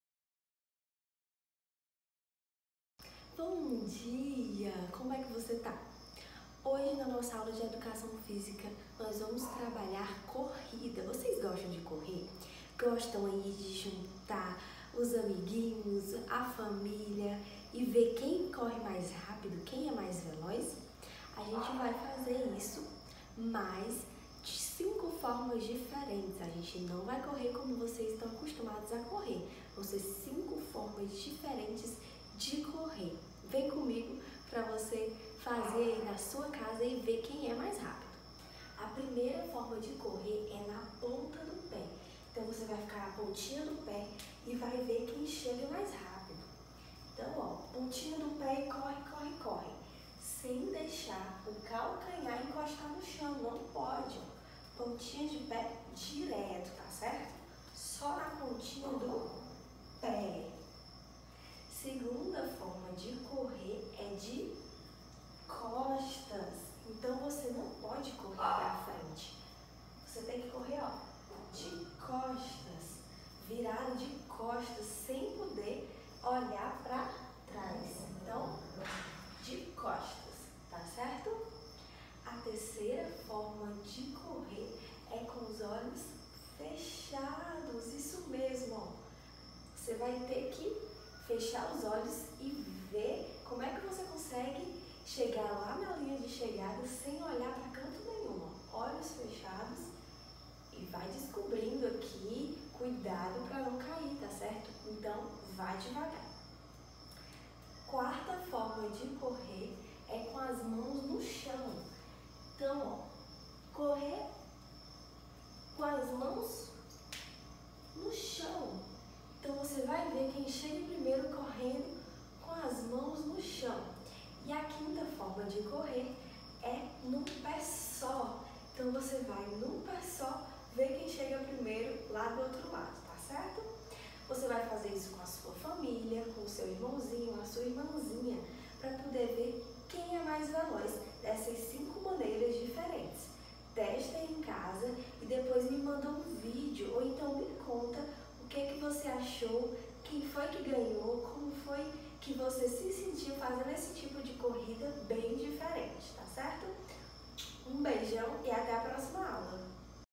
Educação Física - Aula 3
Vamos correr de um jeito diferente? Olha que legal esses 5 tipos de corrida super desafiadoras 😍